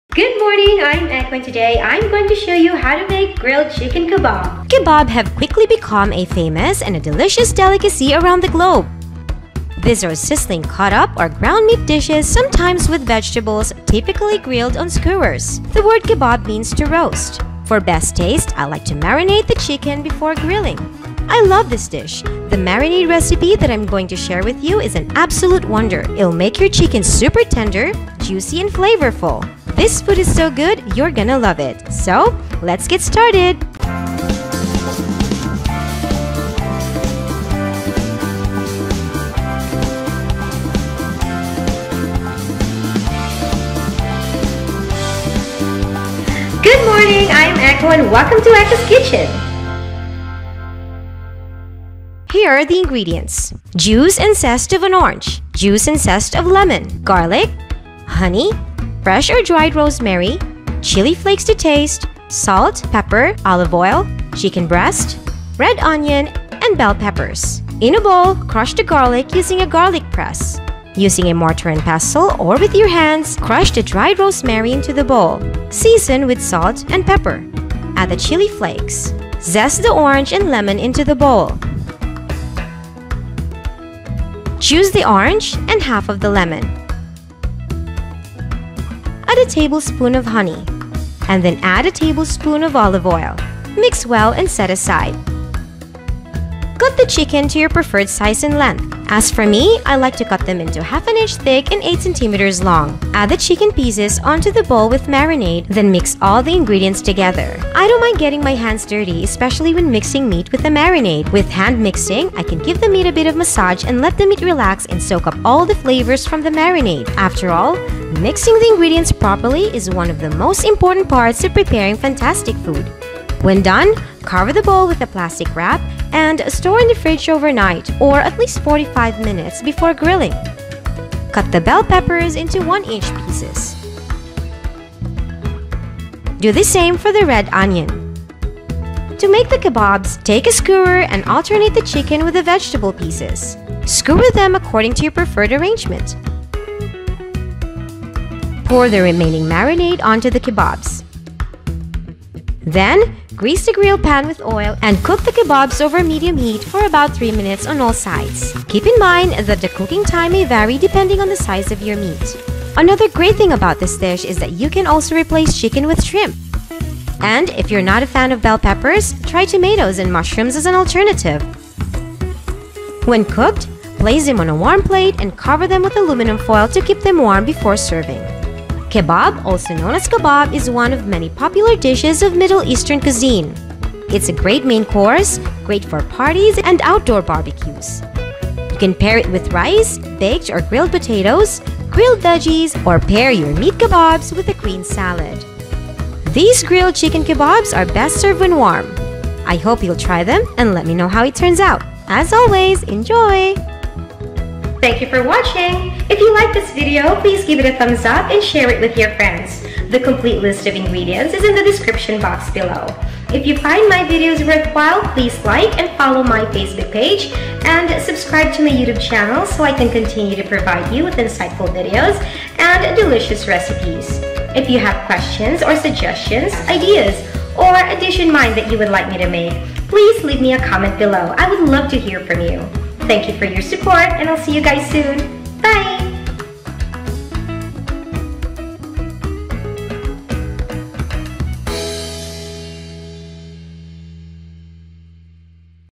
Grilled Chicken Kebab | Chicken Recipe
v29 | Grilled Chicken Kebab by Echo's Kitchen 😋

Good morning!
For today's cooking vlog, I'm going to share with you my Grilled Chicken Kebab recipe.
These are sizzling, super savoury, super tender and juicy chicken on skewers marinated in citrus juice with garlic and rosemary.

INGREDIENTS:
• Juice of 1 medium size Orange
• Orange and Lemon Zest
• Juice of ½ Fresh Lemon
• 4 Cloves of Garlic
• 1 Tbsp of Honey or Sugar
• 1 Tbsp of Fresh Rosemary or 1-2 tsp Dried Rosemary
• Chili Flakes, to taste
• Salt & pepper, to taste
• 2 Tbsp of Olive Oil, divided
• 400g Boneless Skinless Chicken Breast, 1/2 inch thick 8 cm length
• Red Onion, cut into 1 inch pieces
• Bell Peppers, cut into 1 inch pieces

Hope you'll try it and let me know how it turns out.

Cheers!❤️
Echo Caceres